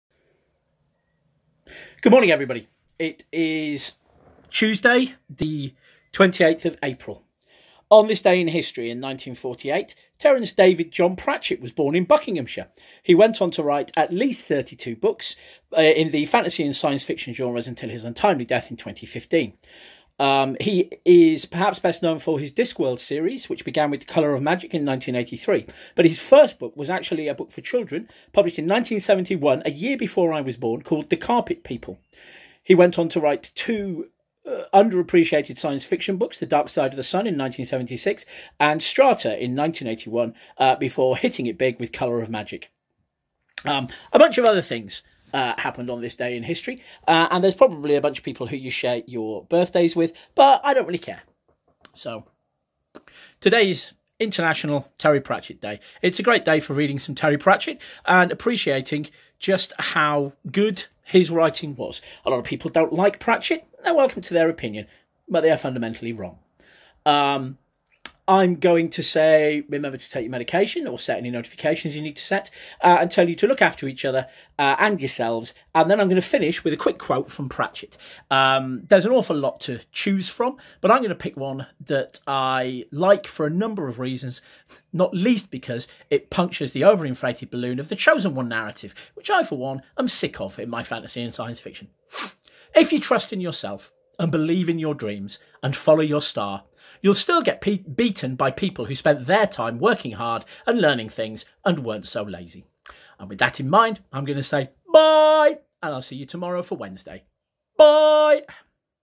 Tuesday 28th April
It's Tuesday 28th April! It's International Terry Pratchett day - or it would be if there as any justice.